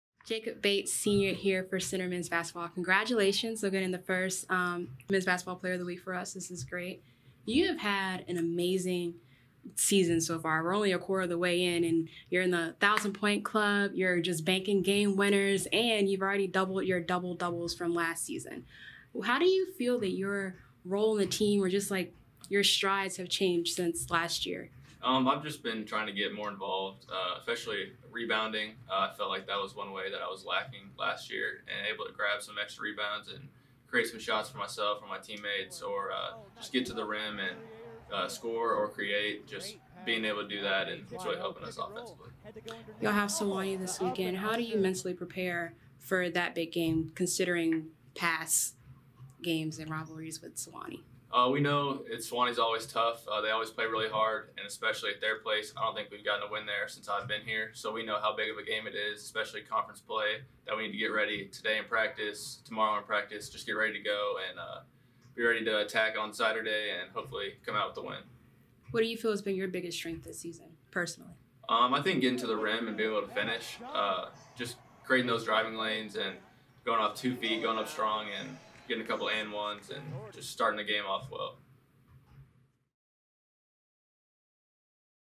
SAA Player of the Week: Jacob Bates
Hear from senior and SAA Player of the Week Jacob Bates on his successful season so far and getting ready to face Sewanee this weekend in Tennessee!